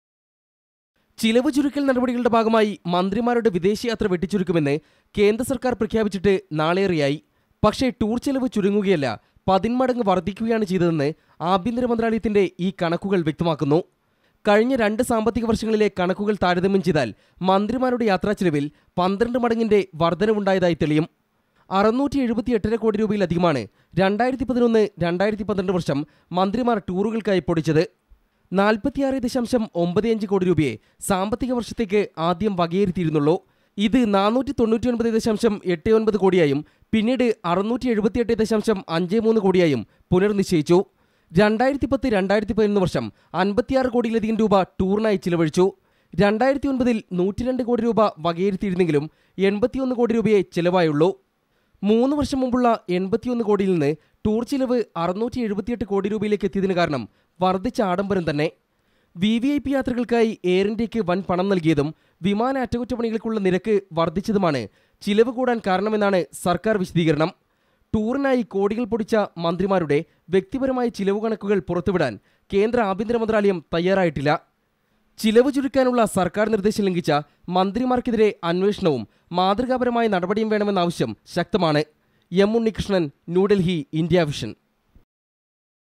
Ministers Travel Expenses Increases 12 Fold This Financial Year
കേന്ദ്രസര്‍ക്കാരിന്റെ ചെലവ് ചുരുക്കല്‍ നടപടികള്‍ക്കിടയിലും മന്ത്രിമാരുടെ യാത്രാചെലവുകളില്‍ 12 മടങ്ങ് വര്‍ദ്ധന. കഴിഞ്ഞ സാമ്പത്തികവര്‍ഷം 678 കോടി രൂപയിലധികം മന്ത്രിമാര്‍ ചെലവിനായി പൊടിച്ചു. 2009-ലെ 81 കോടി രൂപയുടെ ചെലവാണ് ഇപ്പോള്‍ ഭീമമായി ഉയര്‍ന്നിരിക്കുന്നത്.